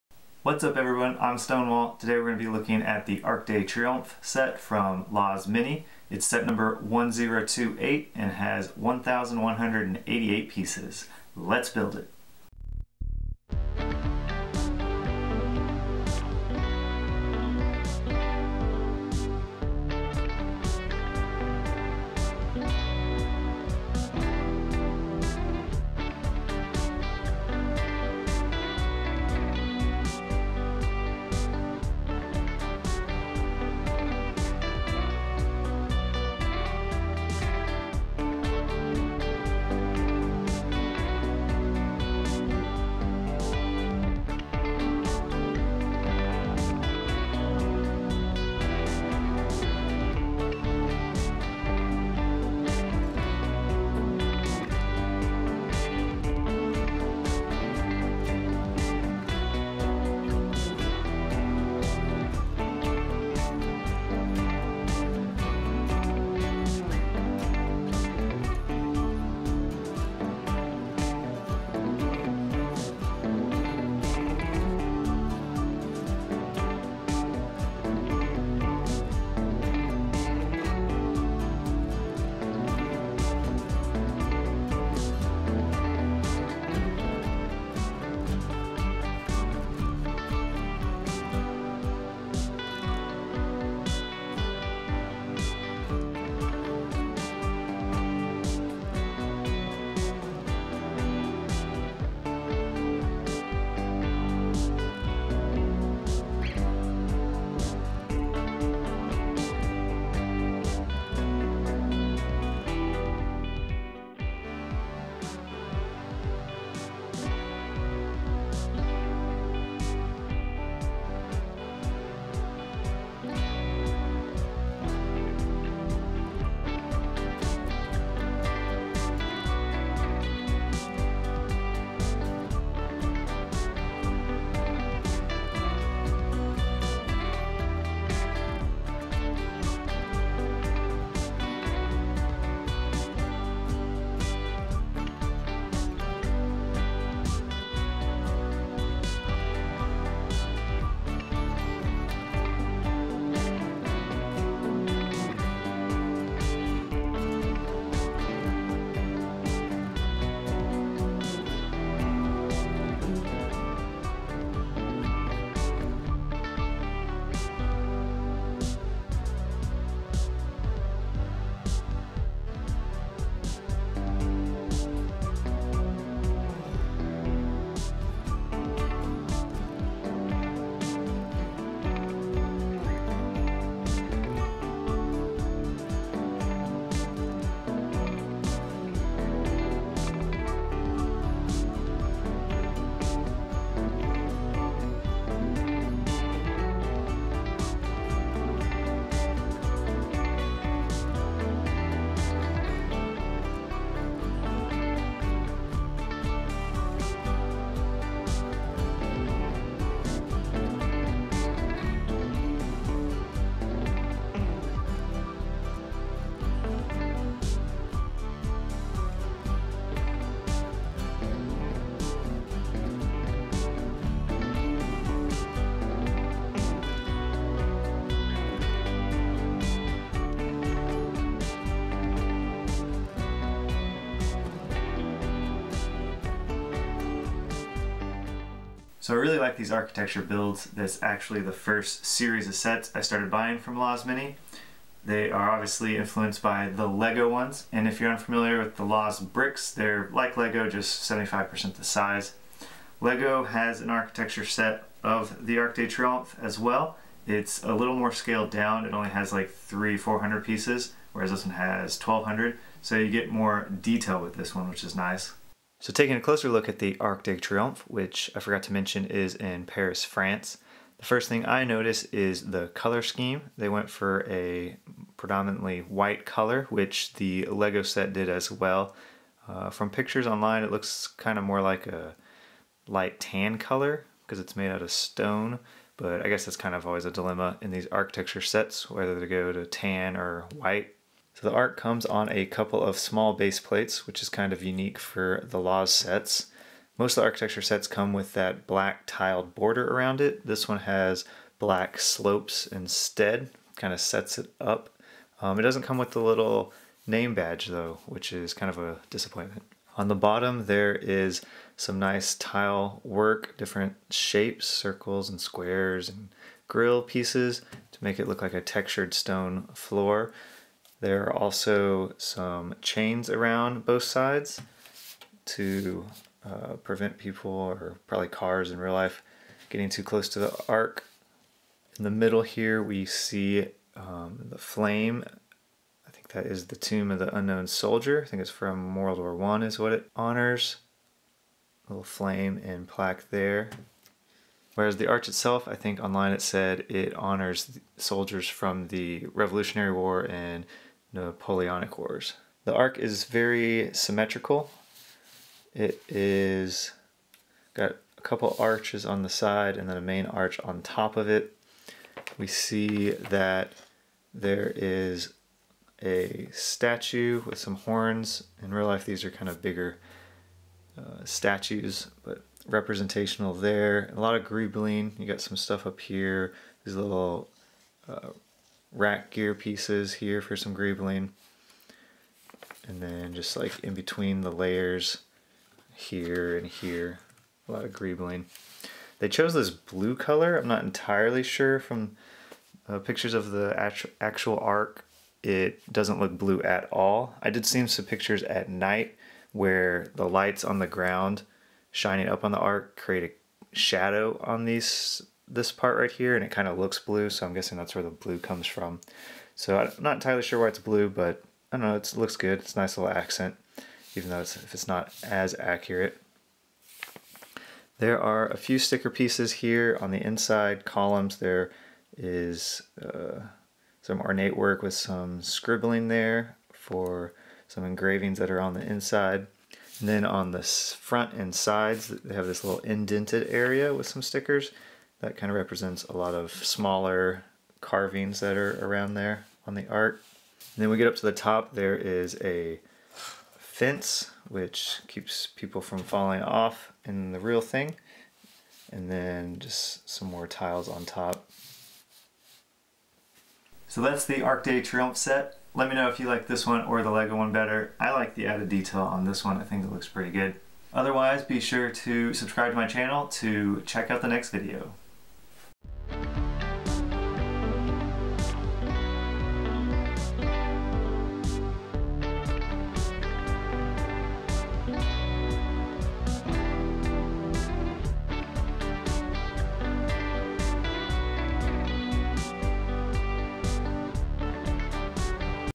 Arc de Triomphe (more detailed) | Speed Build & Review | Architecture Series | Loz 1028 | Lego -ish
Here's a speed build timelapse and review of the Arc de Triomphe set from the Loz Blocks Architecture series.  It's set number 1028 and has 1,188 pieces.  This set is larger and more detailed than Lego's version.

Loz 俐智积木

This is my preferred Aliexpress store